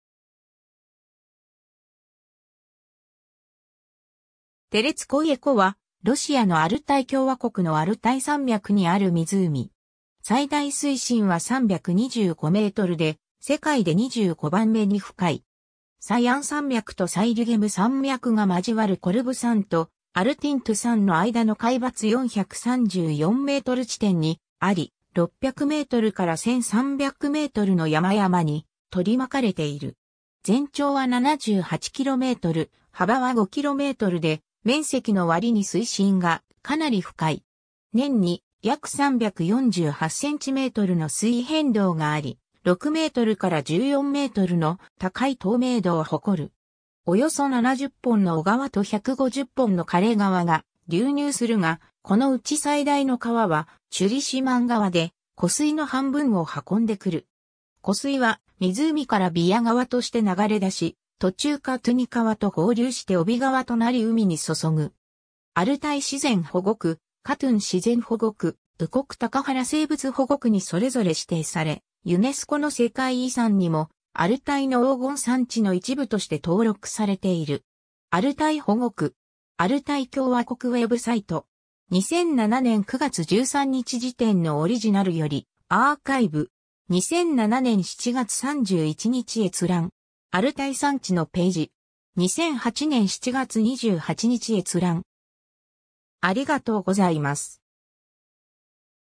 テレツコイェ湖
テレツコイェ湖（テレツコイェこ、英: Lake Teletskoye、ロシア語: Телецкое озеро）は、ロシアのアルタイ共和国のアルタイ山脈にある湖。
最大水深は325mで、世界で25番目に深い。
 サヤン山脈とサイリュゲム山脈が交わるコルブ山とアル＝ティントゥ山のあいだの海抜434m（ 北緯51度31分45秒 東経87度42分53秒 / 北緯51.52917度 東経87.71472度 / 51.52917; 87.71472）地点にあり、600mから1300mの山々に取り巻かれている。
全長は78km、幅は5kmで、面積の割に水深がかなり深い。
年に約348cmの水位変動があり、6mから14mの高い透明度をほこる。
 およそ70本の小川と150本の涸れ川が流入するが、このうち最大の川はチュリシュマン川で、湖水の半分を運んでくる。
湖水は湖からビヤ川として流れ出し、途中カトゥニ川と合流してオビ川となり海に注ぐ。
 アルタイ自然保護区、カトゥン自然保護区、ウコク高原生物保護区にそれぞれ指定され、ユネスコの世界遺産にも「アルタイの黄金山地」の一部として登録されている。
 ^ “アルタイ保護区”. アルタイ共和国ウェブサイト. 2007年9月13日時点のオリジナルよりアーカイブ。
2007年7月31日閲覧。
 ^ “アルタイ山地のページ”. 2008年7月28日閲覧。